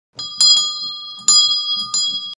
Türglocke
Türglocke mit Hufeisengestell. Bronzierte Glocke 10 cm Durchmesser